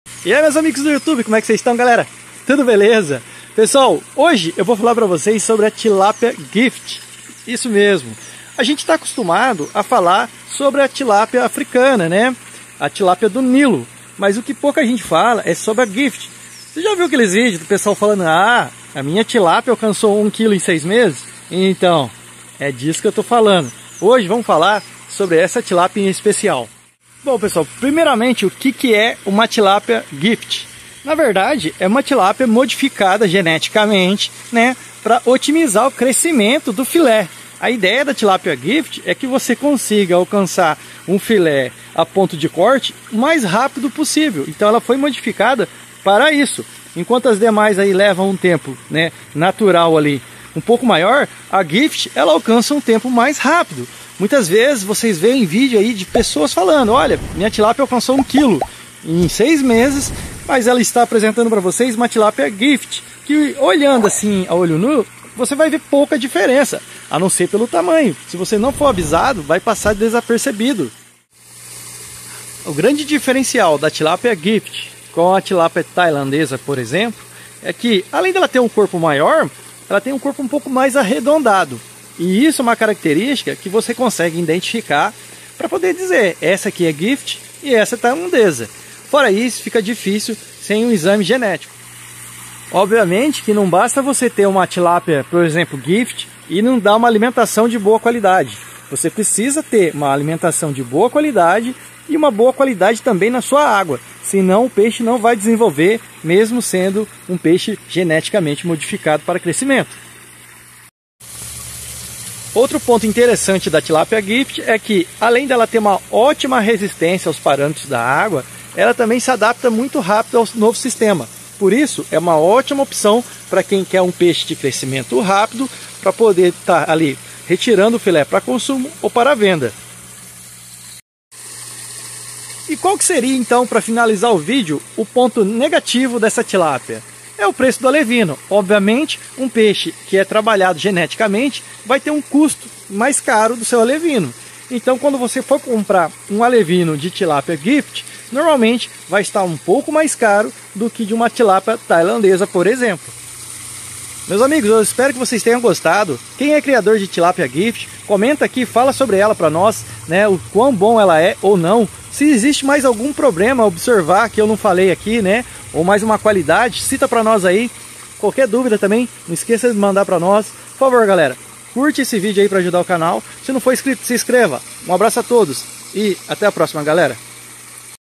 Qual a tilápia que cresce mais rápido ?
Neste vídeo falo sobre a tilápia gift, um peixe modificado geneticamente para poder crescer mais rápido.